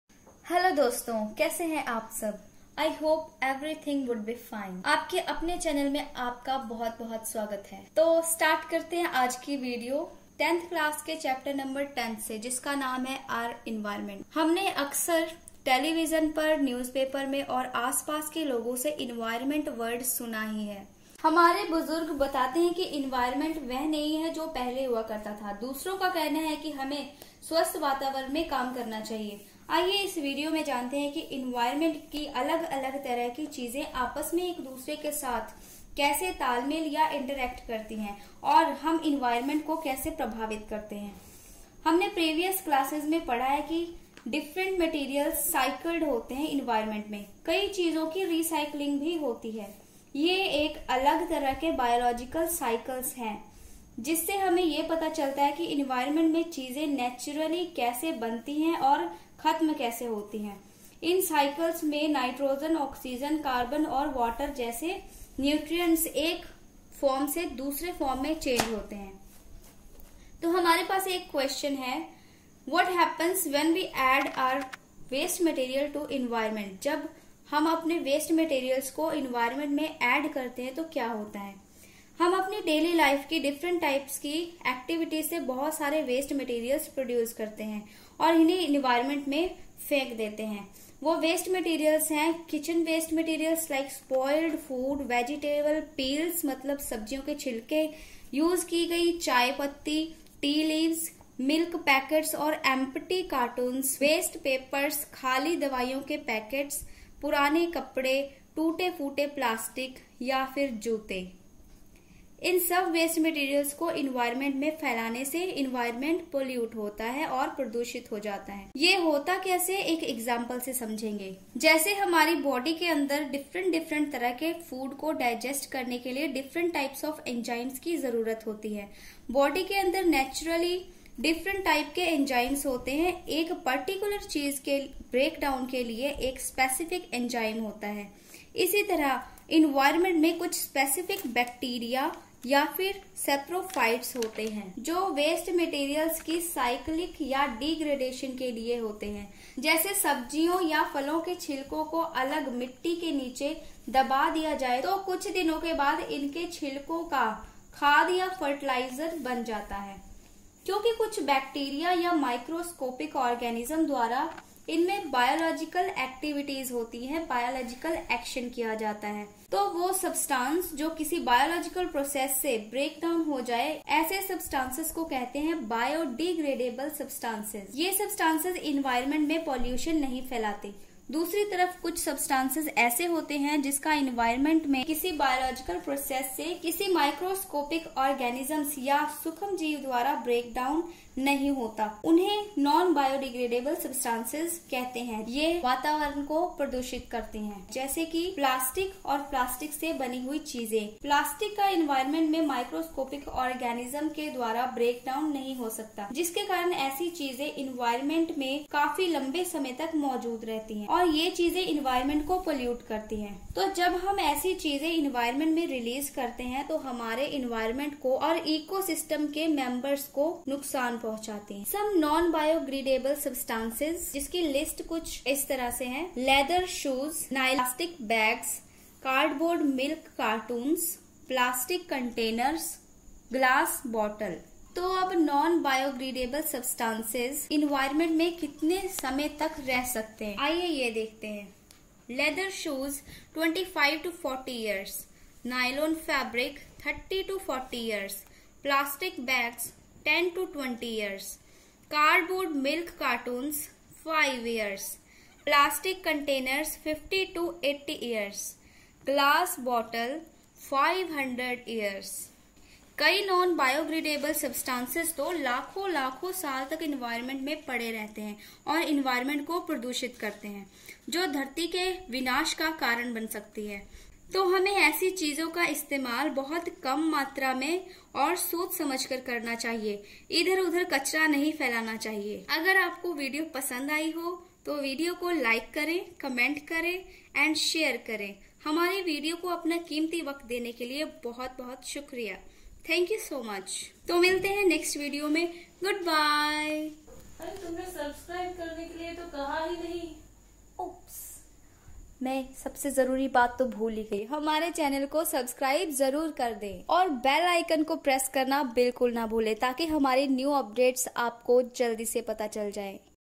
Our Environment - GURUKUL - Part-1 | class 10 science intro.full explanation in Hindi
Hey!!
This is Anjali sharma.
Lets do the 15 chapter our environment from class 10
Our Environment in hindi

Our Environment class 10
Our Environment class 10 science
Our Environment explanation